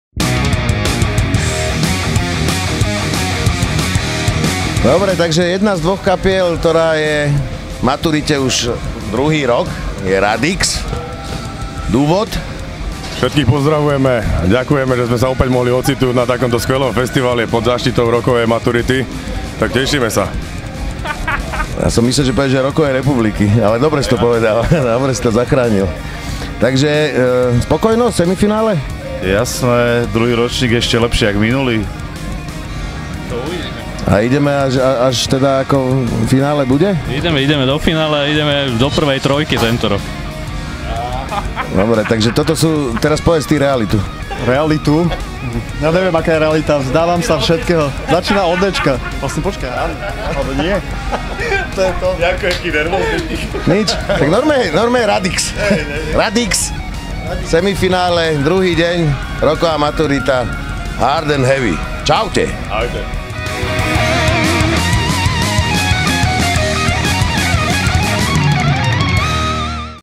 ROCKOVÁ MATURITA 2016 - RADIX
. krátky rozhovor z Topfestu .